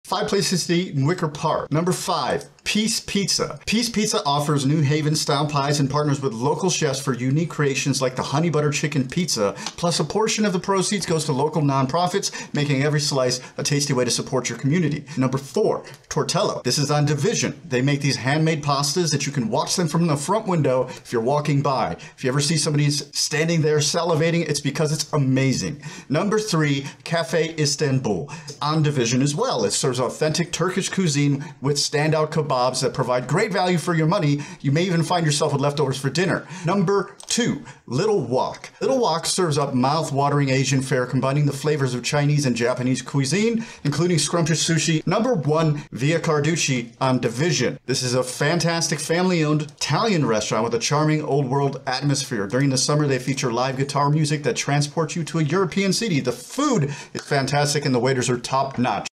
Best 5 Restaurants To Eat In Wicker Park Chicago Illinois
Wicker Park, Chicago, is known for its vibrant food scene, with a range of options from classic Italian to fresh Asian cuisine. Here are five top-notch restaurants in the area you don't want to miss.

Via Carducci: This charming, family-owned Italian restaurant is a true gem of Wicker Park. With an old-world atmosphere and top-notch service, you'll feel like you've been transported to Europe. The menu features delicious Italian dishes, and during the summer, live guitar music adds to the authentic experience.

Little Wok: This Asian eatery combines the flavors of Chinese and Japanese cuisine to create mouth-watering dishes that you won't be able to resist. From sushi to stir-fry, Little Wok is sure to please any palate.

Café Istanbul: This Division Street restaurant serves up authentic Turkish cuisine, with standout kebabs that offer great value for your money. You'll find yourself wishing you could eat here every day!

Tortello: This handmade pasta restaurant on Division Street is a must-visit for any pasta lover. Watch the masters at work through the front window, and be prepared to drool as the aroma of fresh pasta wafts out.

Piece Pizza: New Haven-style pies are the specialty at Piece Pizza, and the restaurant partners with local chefs to create unique creations like the Honey Butter Chicken Pizza. Plus, a portion of proceeds goes to local nonprofits, making every slice a tasty way to support your community.

These are just a few of the many amazing places to eat in Wicker Park. Whether you're looking for a romantic evening out, a casual lunch, or a family dinner, Wicker Park has you covered with its diverse and delicious food scene. So why wait? Head out and discover the best restaurants in Chicago today!